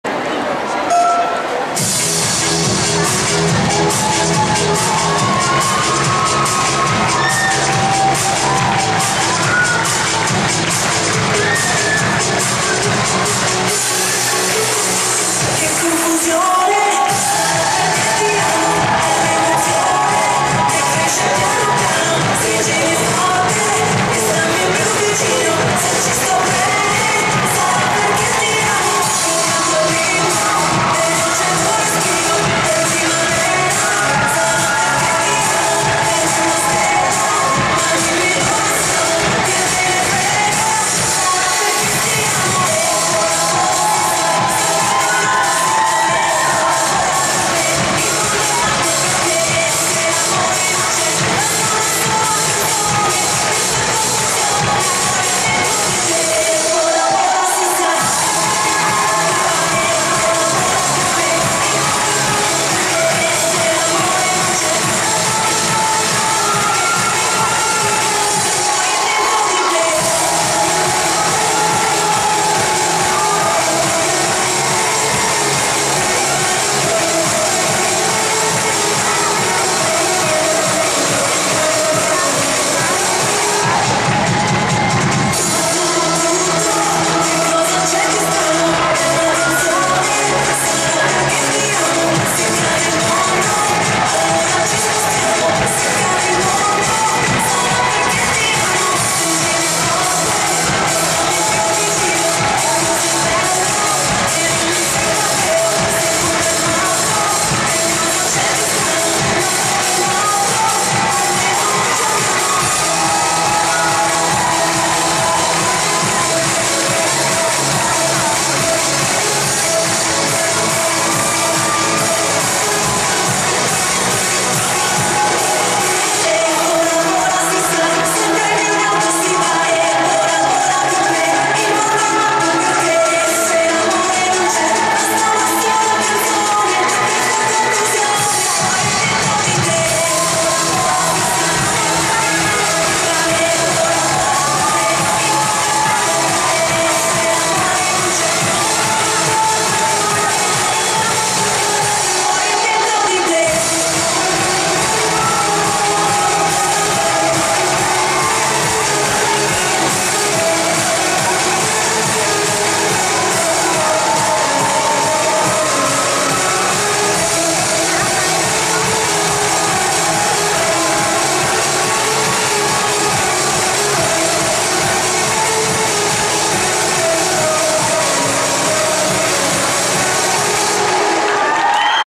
Mažoretky Minipanenky Hranice - Podhostýnský pohár
1.místo - baton kadetky
baton, Czech republic